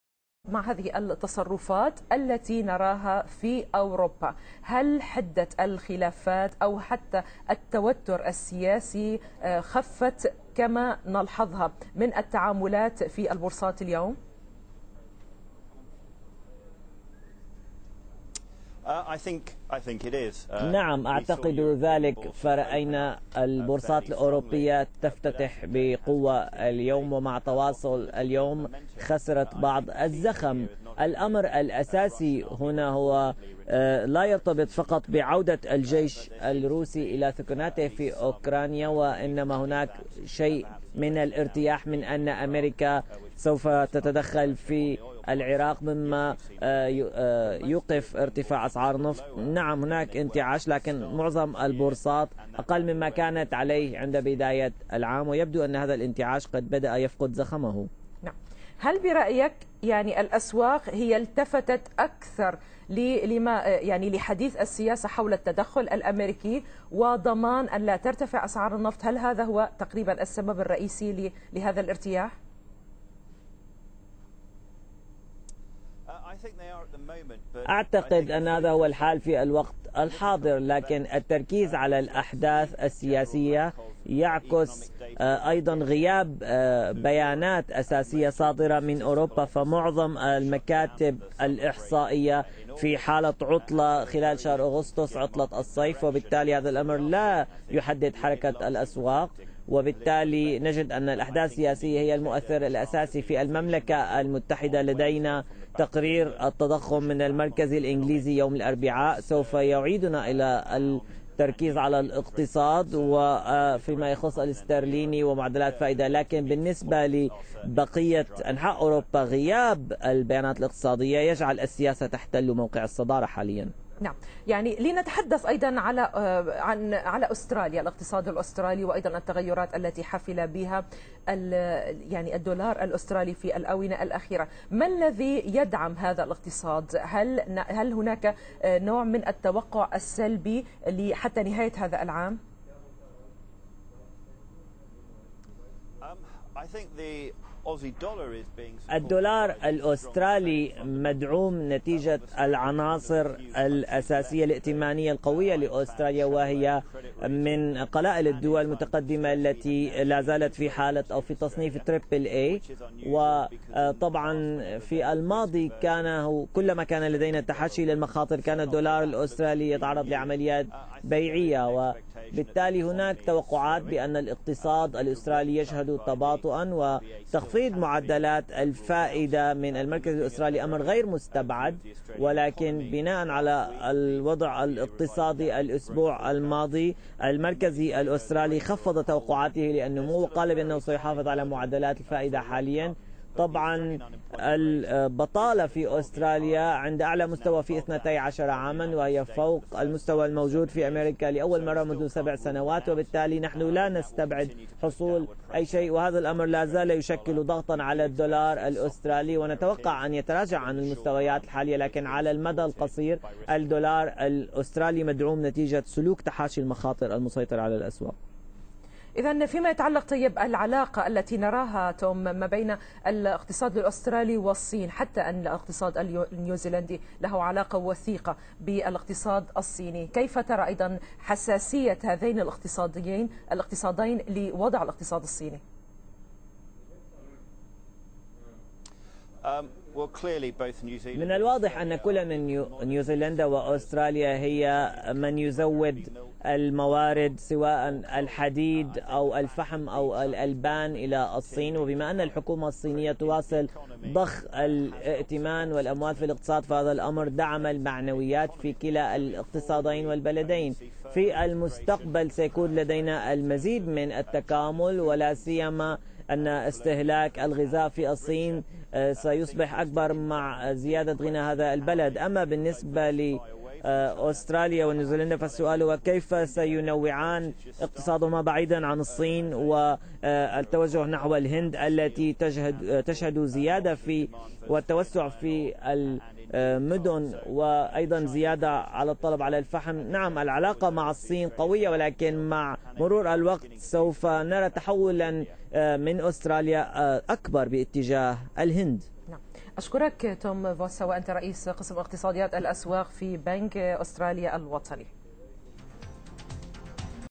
التوترات الجيوسياسية تقود حركة الاسهم الاوروبية
قال رئيس قسم اقتصاديات الاسواق الاوروبية في بنك استراليا الوطني توم فوسا بأن غياب البيانات الاقتصادية الاساسية في اوروبا حاليا نتيجة العطلة الصيفية جعل التوترات الجيوسياسية هي المحرك الاساسي للاسواق.

وأضاف فوسا أن الاقتصاديين الاسترالي والنيوزلندي بدء يتجهان بوتيرة اكبر نحو الهند مبتعدين عن الصين بهدف تنويع نشاطهم الاقتصادي مع دول اخرى.